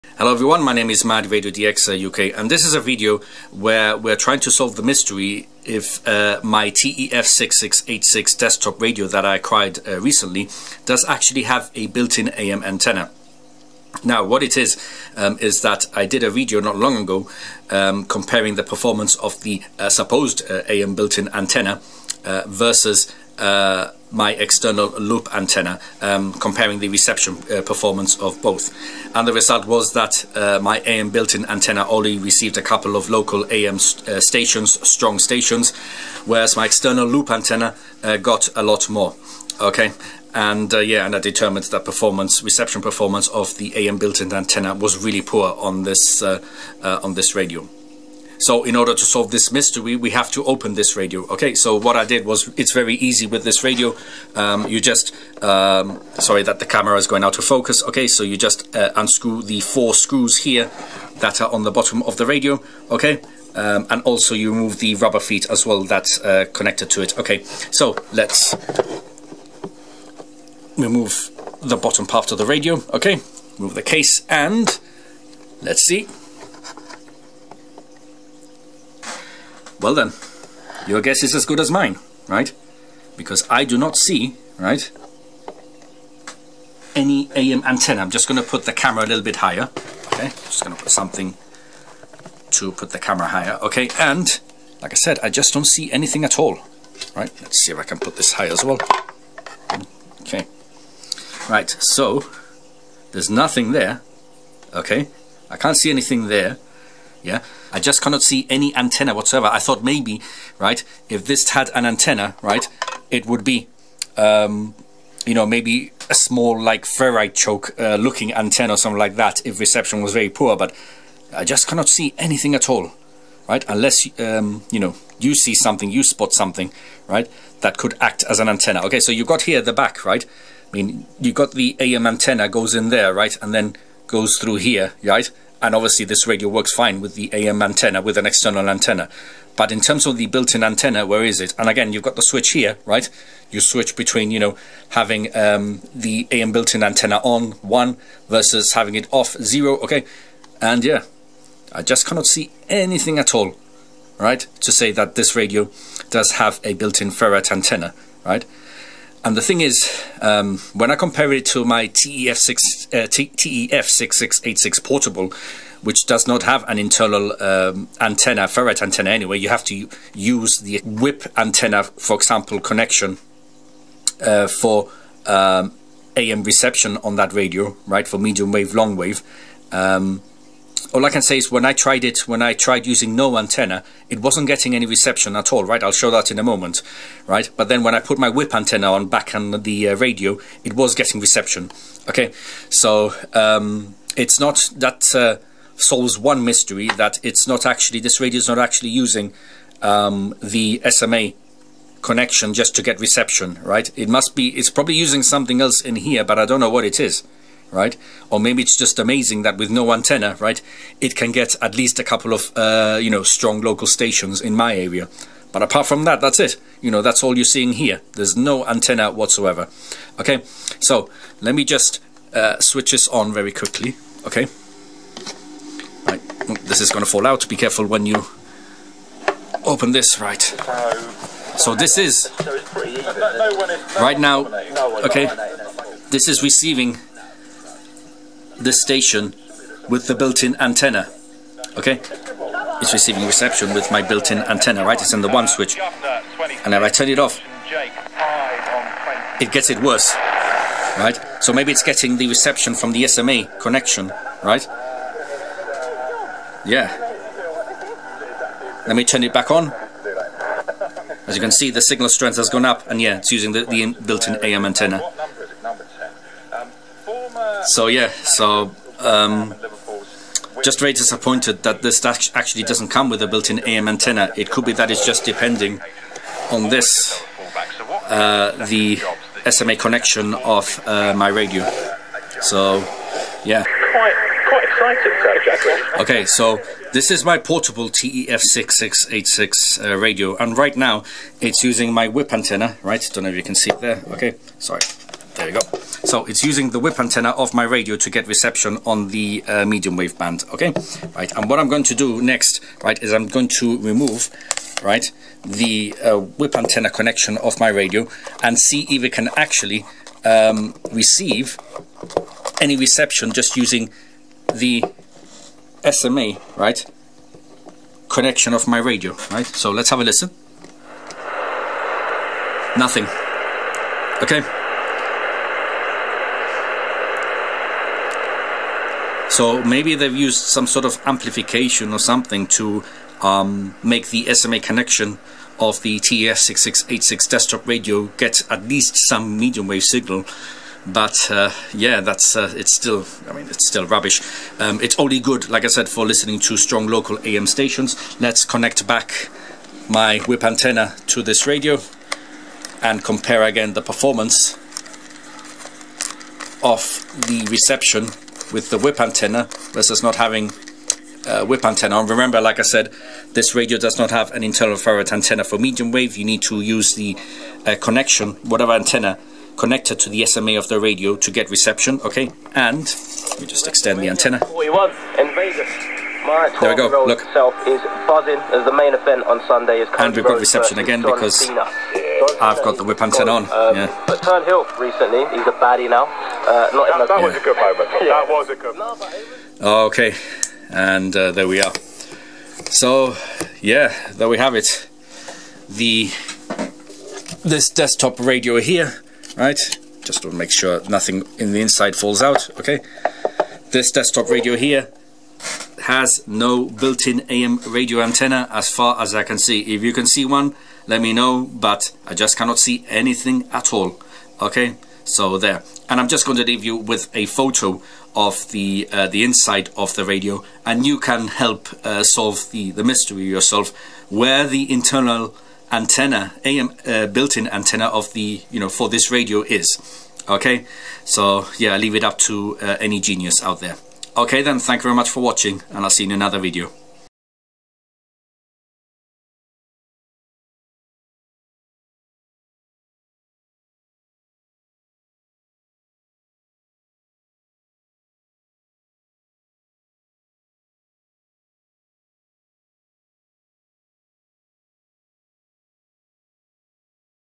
Does my TEF 6686 Desktop have an AM built in antenna? Looking inside the radio
Update: Thanks to Paul Cobley for his reply which has solved the mystery of the AM antenna inside this radio. Quoting from his comment, "inside the radio there is a rectangle printed circuit board near the back this is the am antenna. It works like a loop antenna."
Original description: Trying to solve the mystery if this TEF 6686 Desktop radio has an AM built in antenna. This has been puzzling me after I did a video doing a reception performance comparison on Medium Wave of the supposed AM built in antenna vs an external loop antenna.

00:00 Introduction
00:14 Explanation
00:52 How to open the radio
01:14 Looking inside the radio
02:10 SMA connection & AM antenna switch
02:43 Guessing how it gets AM reception
03:50 Comparing AM built in antenna on vs off
04:44 TEF 6686 Portable internal AM comparison
06:33 Closing & thoughts
07:17 Photo of inside the radio